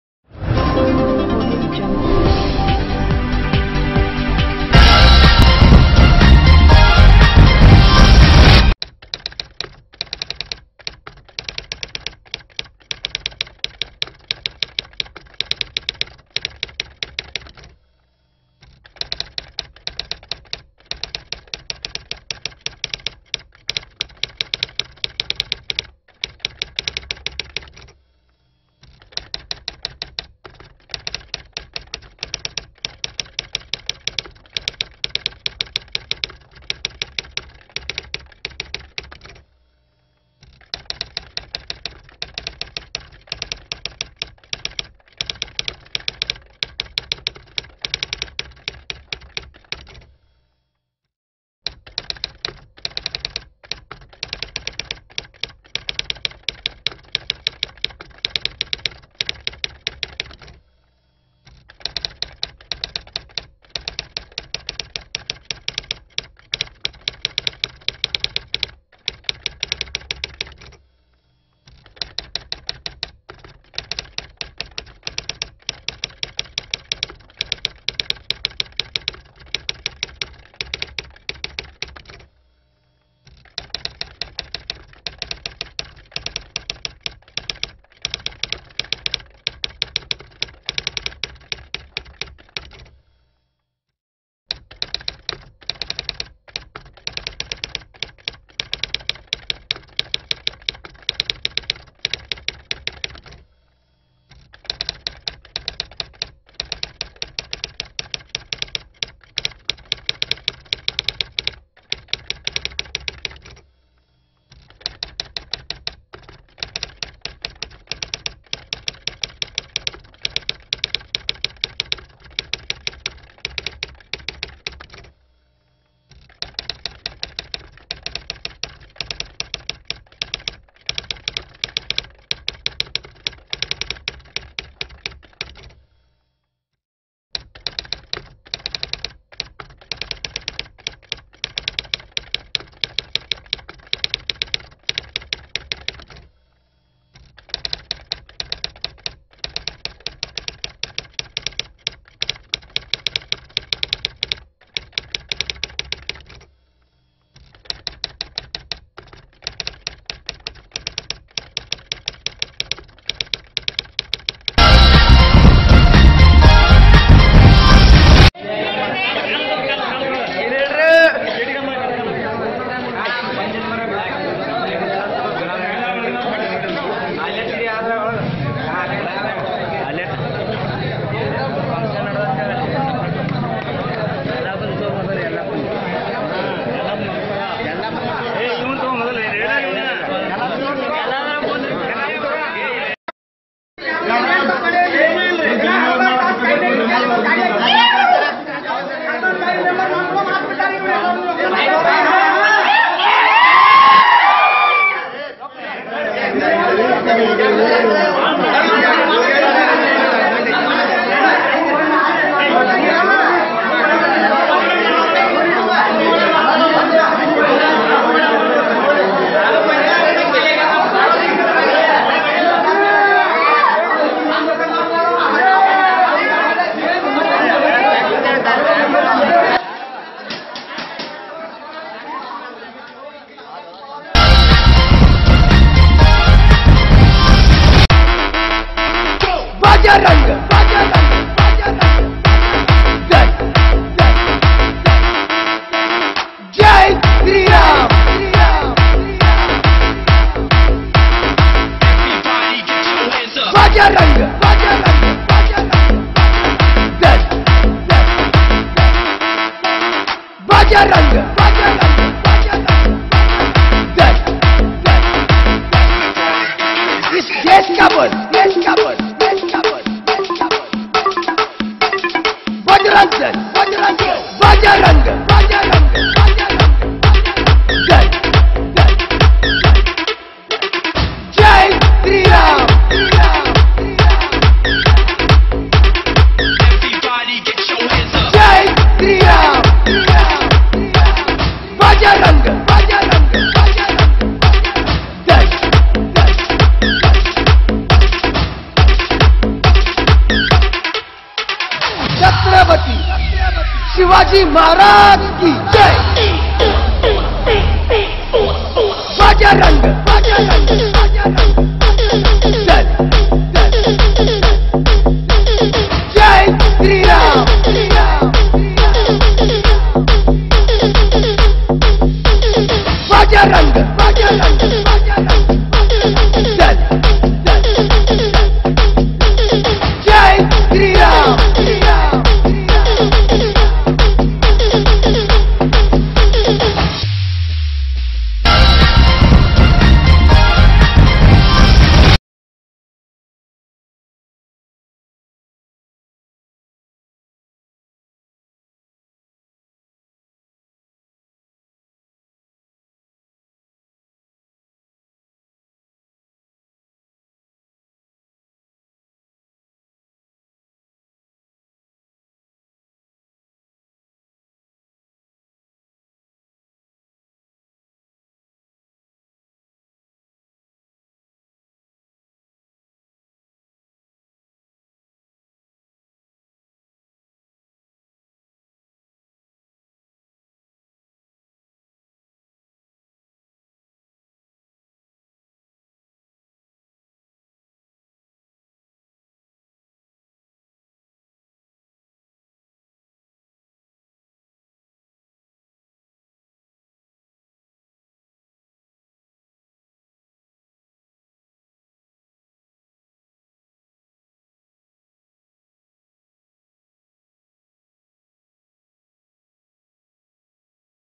VAASTAV NEWS VIJAYAPUR
ಪೋಲೀಸ್ ಪೋಲೀಸರಿಗೆ ದ್ರೋಹ  ಮಾಡಿದರೆ ಸಾಮಾನ್ಯ ಜನರ ಗತಿಯೇನು